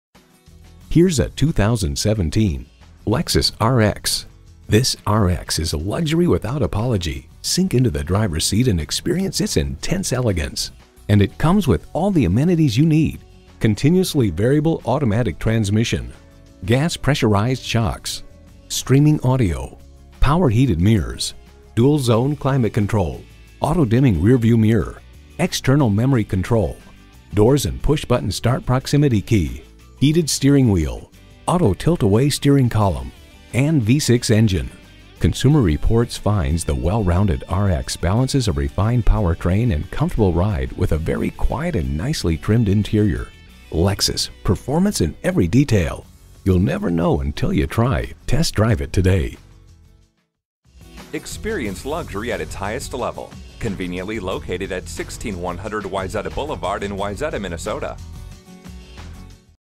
Used 2017 Lexus RX Minneapolis, Minnetonka, MN
Year: 2017
Make: Lexus
Model: RX
Trim: RX 450h AWD
Engine: 213 L V6 Cyl.
Transmission: Continuously Variable Trans
Color: Nebula Gray Pearl
Interior: Black
Mileage: 11407
Stock #: 22780P
VIN: 2T2BGMCAXHC014162
L/CERTIFIED BY LEXUS! 1 Owner! Equipped with Navigation, Back Up Camera, Blind Spot Monitor, Heated & Ventilated Seats, Heated Steering Wheel, Moonroof, Bluetooth, 20 Wheels and so much more! L/Certified Warranty is good until 6/21/2023 with Unlimited Mileage! Also included is the 2 Year/20,000 mile Complimentary Scheduled Maintenance Program on Every L/Certified Lexus! See Lexus of Wayzata for Details!

Sunday Closed
Address:
16100 Wayzata Blvd